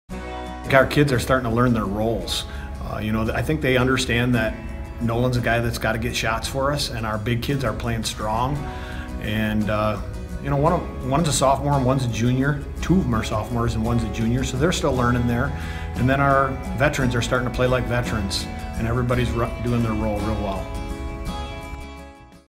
Hemlock basketball coach Matt Bauer talks about back-to-back wins
The Hemlock boys basketball team finished off a strong week Friday with a win over Michigan Lutheran Seminary after beating Sanford Meridian Tuesday.